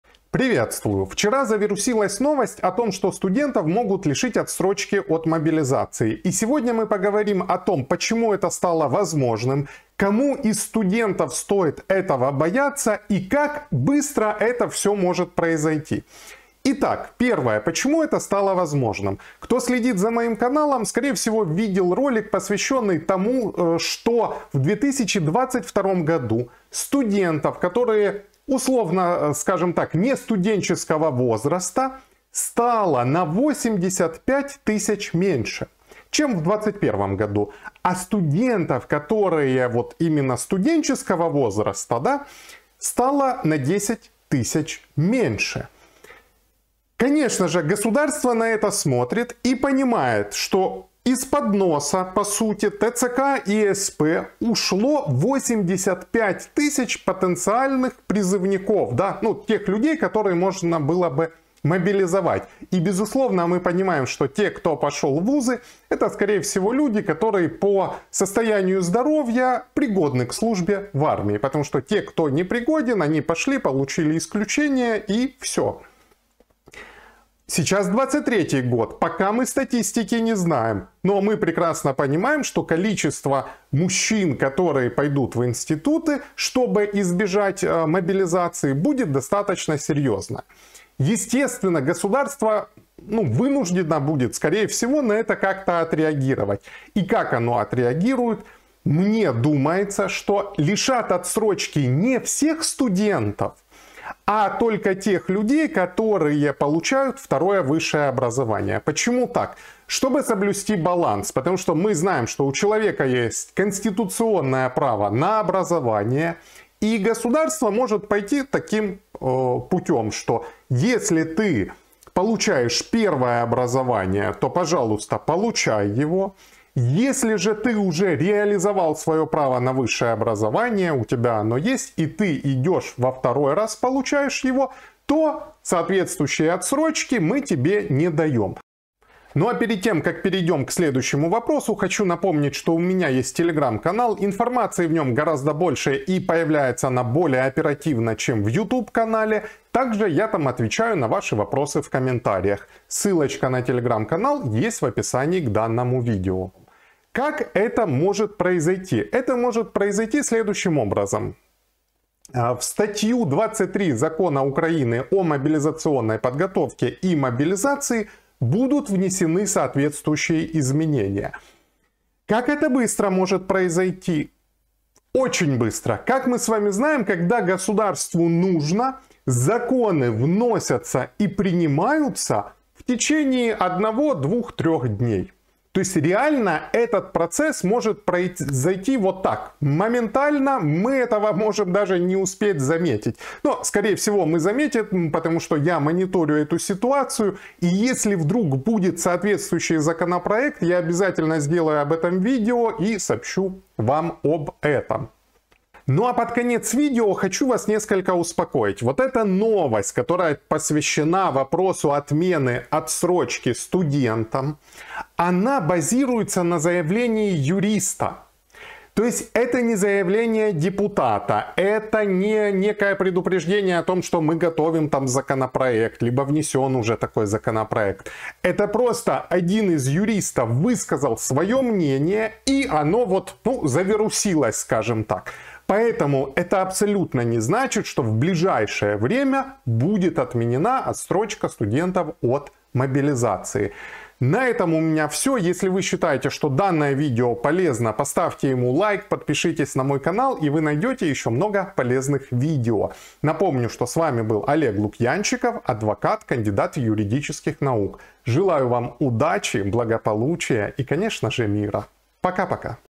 Отменят отсрочку студентам?
Новость о возможной отмене отсрочки студентов от мобилизации в Украине расстроила многих людей. Многие задаются вопросом: а не ограничивают ли таким образом право на образование? А возможно ли это делать во время военного положения? Да, это возможно. И для этого нужно лишь изменить закон Украины про мобилизацию.